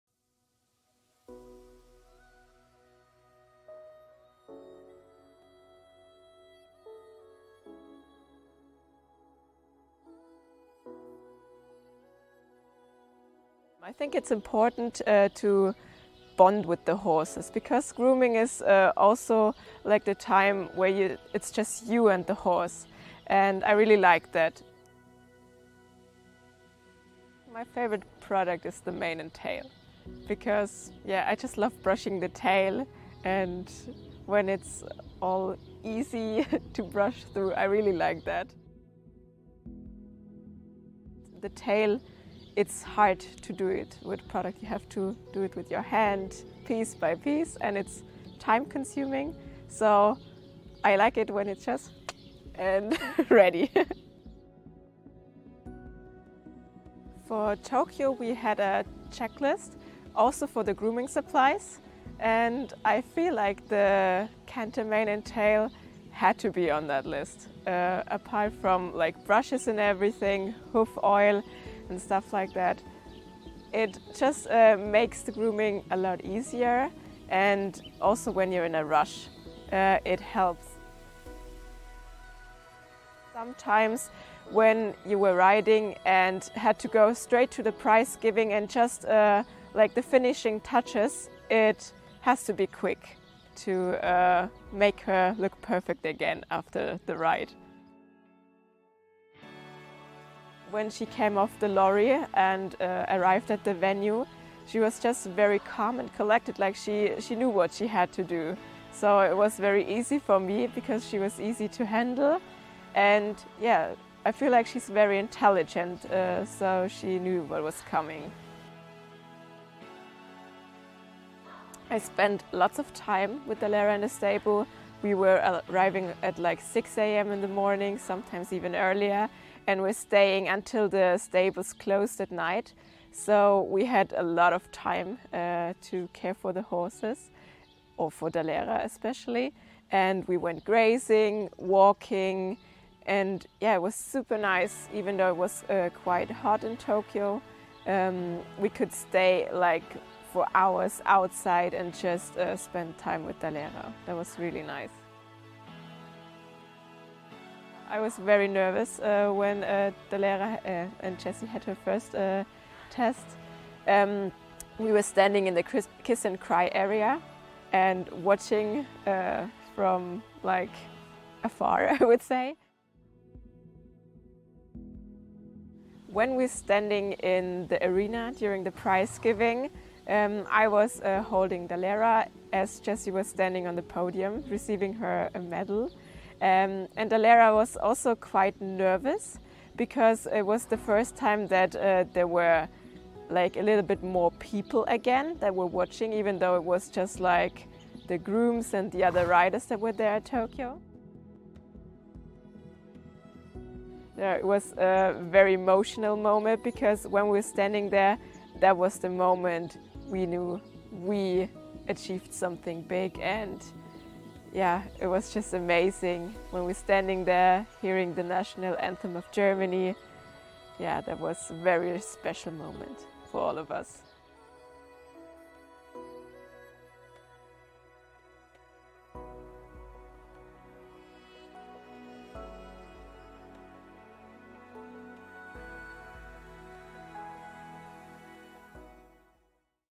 GOLDEN MOMENTS AT TOKYO OLYMPICS WITH PAULA KALLMUNZER
We caught up with PAULA KALLMUNZER  during our recent trip to Aubenhausen to discuss all things grooming gold and her favourite memories from her experience grooming for double Olympic Champion Jessica von Bredow Werndl & Dalera at Tokyo!